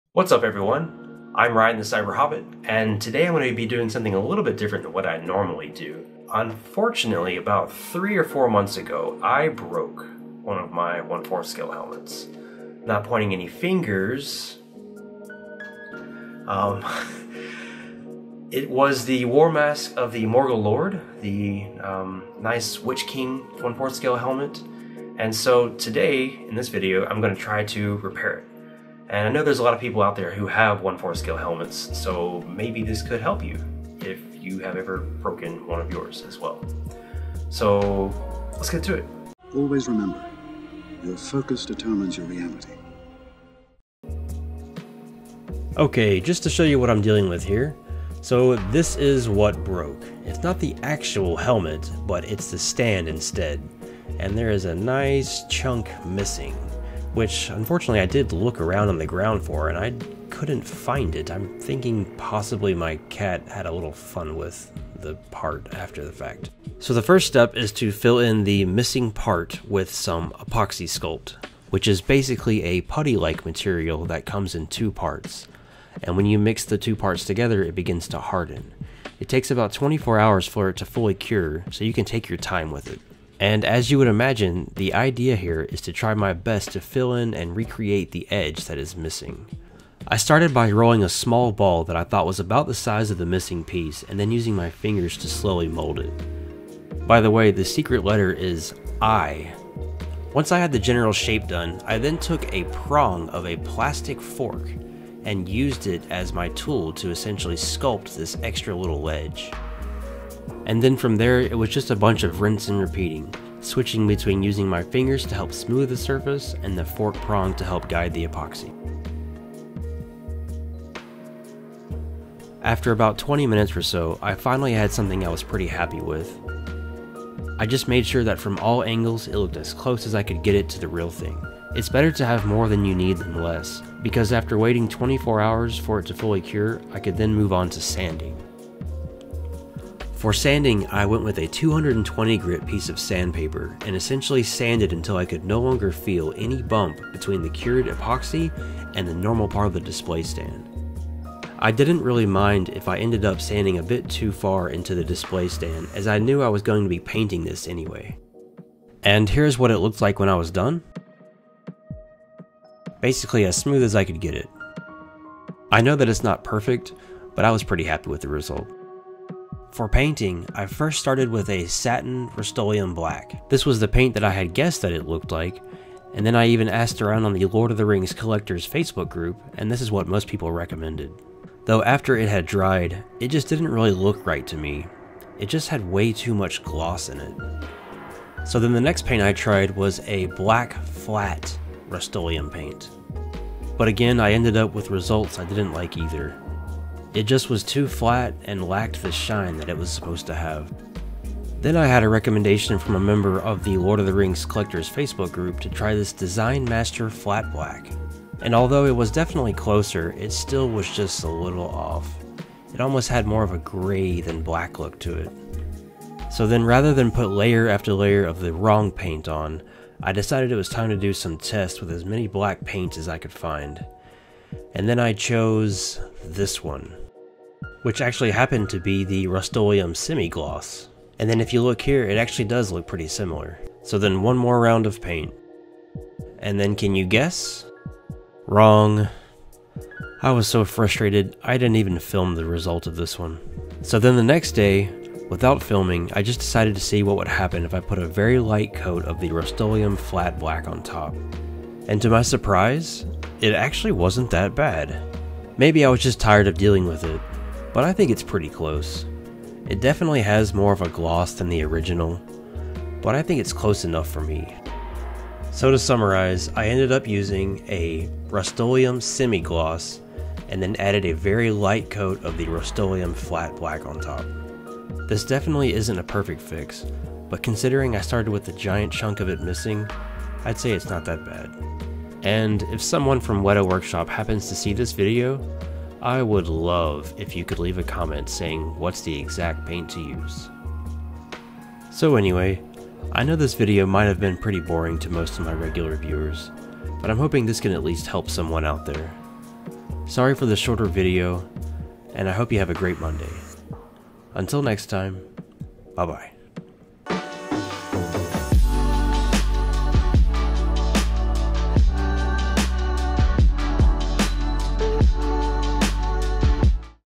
Fixing a Weta Workshop Lord of the Rings 1/4 scale Helmet
In this video I attempt to fix the stand to one of my Lord of the Rings 1/4 scale helmets that unfortunately  broke. I know my end result isn't perfect but it's definitely better than a giant chunk missing.